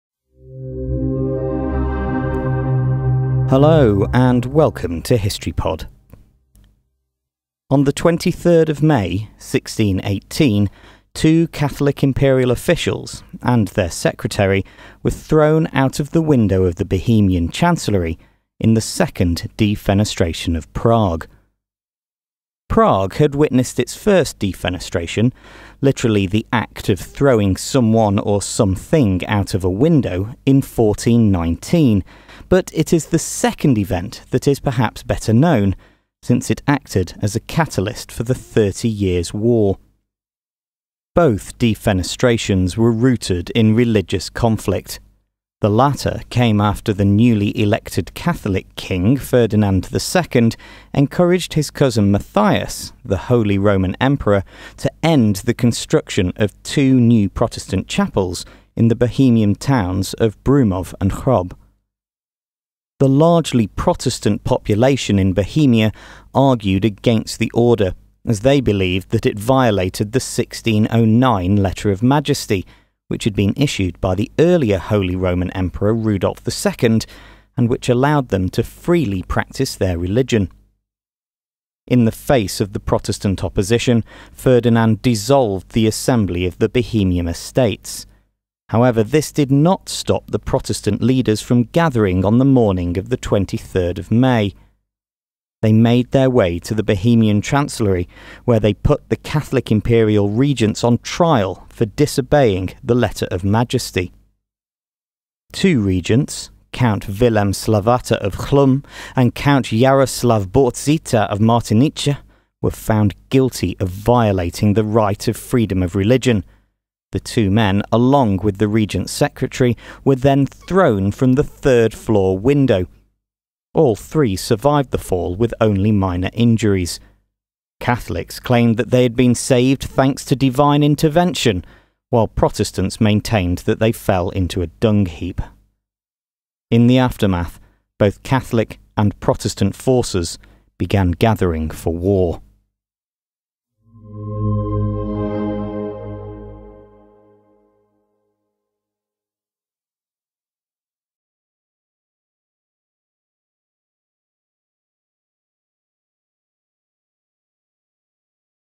23rd May 1618: The Second Defenestration of Prague
Prague had witnessed its first defenestration (literally ‘the act of throwing someone or something out of a window’) in 1419, but it is the second event that is perhaps better known since it acted as a catalyst for the Thirty Years War.

Both defenestrations were rooted in religious conflict. The latter came after the newly elected Catholic king, Ferdinand II, encouraged his cousin Matthias, the Holy Roman Emperor, to order the construction of two new Protestant chapels in the Bohemian towns of Broumov and Hrob to stop. The largely Protestant population in Bohemia argued against the order as they believed that it violated the 1609 Letter of Majesty, issued by the earlier Holy Roman Emperor Rudolf II, which allowed them to freely practise their religion.

In the face of the Protestant opposition, Ferdinand dissolved the assembly of the Bohemian estates. However, this did not stop the Protestant leaders from gathering on the morning of 23 May. They made their way to the Bohemian Chancellery where they put the Catholic imperial regents on trial for disobeying the Letter of Majesty.

Two regents – Count Vilem Slavata of Chlum and Count Jaroslav Borzita of Martinice – were found guilty of violating the Right of Freedom of Religion. The two men, along with the regents’ secretary, were then thrown from the third-floor window. They all survived the fall with only minor injuries. Catholics claimed that they were saved thanks to divine intervention, while Protestants maintained that they fell into a dung heap. In the aftermath, both Catholic and Protestant forces began gathering for war.